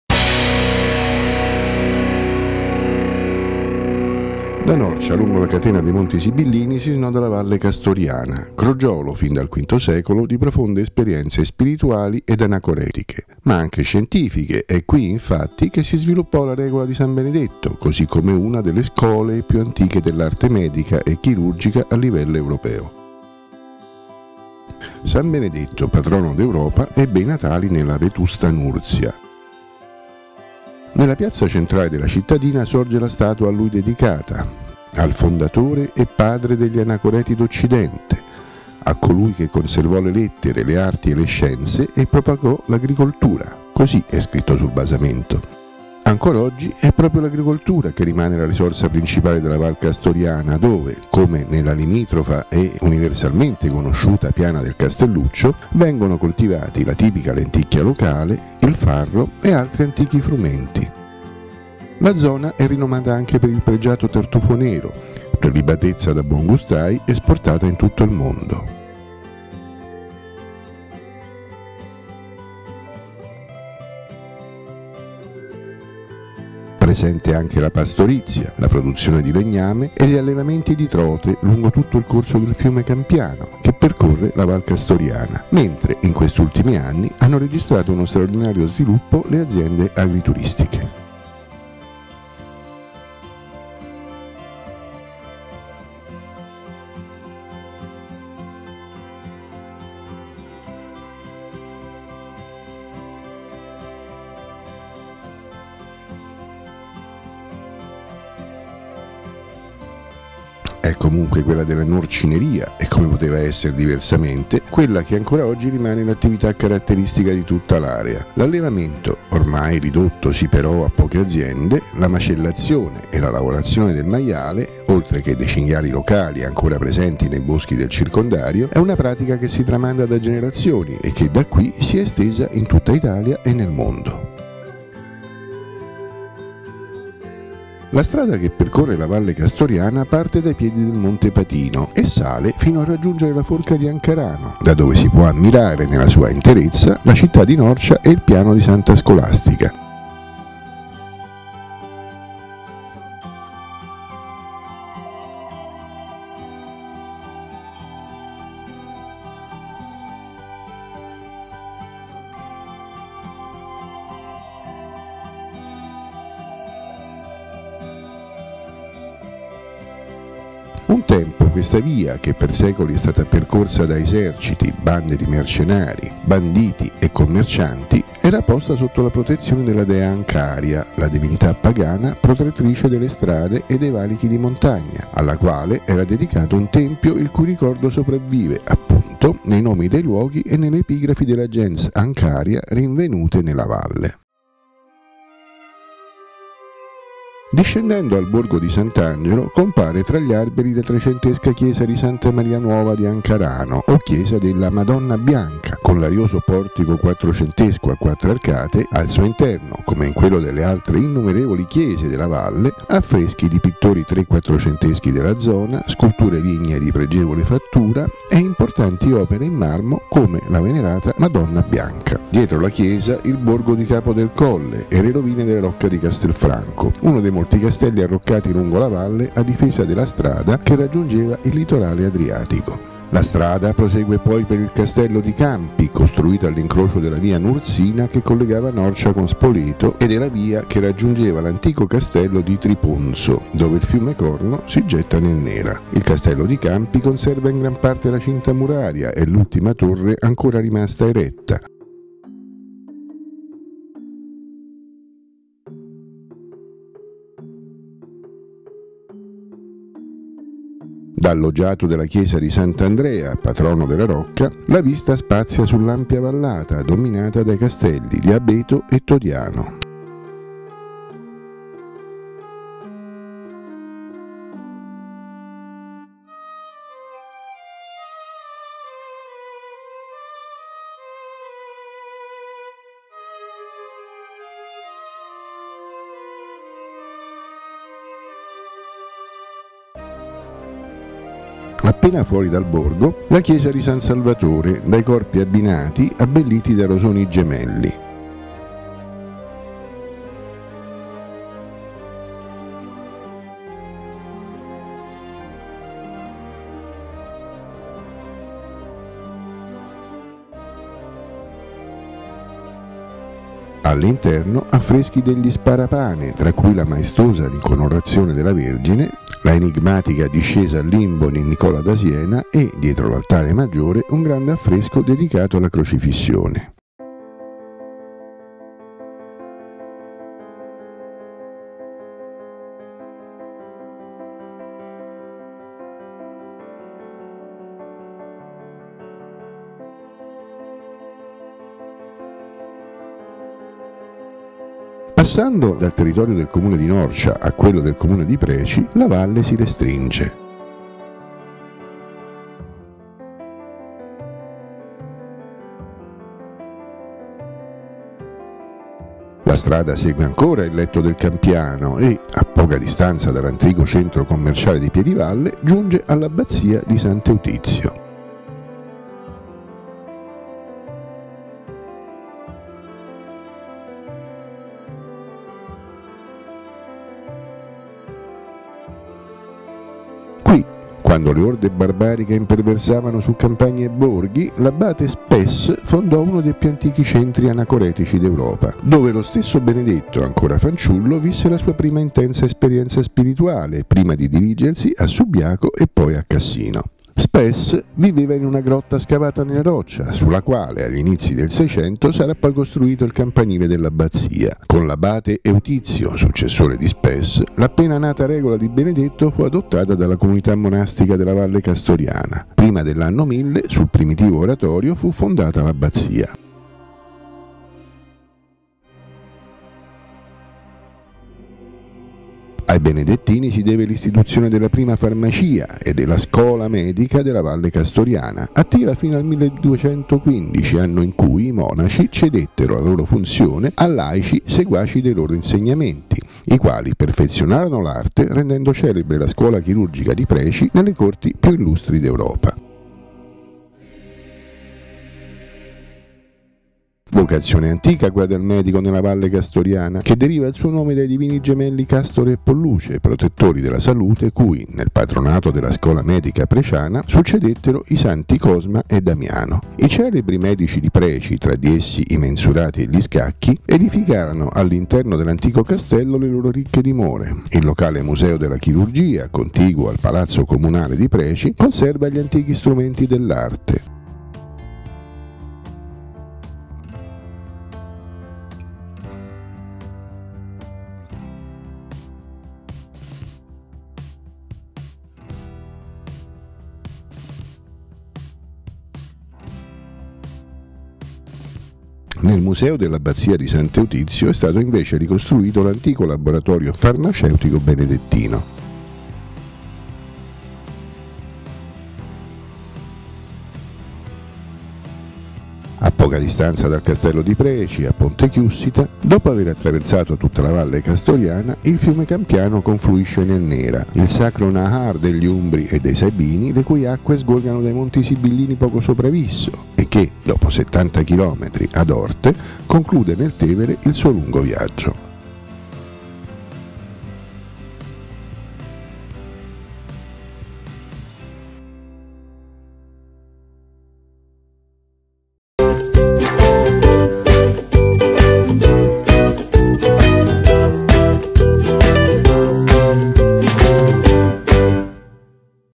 Da Norcia a Preci. Alla scoperta della Val Castoriana
Da Norcia, lungo la catena dei Monti Sibillini, si snoda la Valle Castoriana, crogiolo – fin dal  V secolo – di profonde esperienze spirituali ed anacoretiche, ma anche scientifiche: è qui, infatti, che si sviluppò la “Regola” di San Benedetto, ed è qui che è nata una delle scholae più antiche dell’arte medica e chirurgica a livello europeo. La risorsa principale della valle rimane l’agricoltura, di cui lo stesso Benedetto fu maestro: vi si coltivano la lenticchia, il farro ed altri antichi frumenti. La zona è rinomata per il pregiato tartufo nero, ma è ovviamente la lavorazione del maiale (i “norcini” sono noti in tutto il mondo) l’attività predominante. Grande lo sviluppo, negli ultimi anni, delle aziende agrituristiche.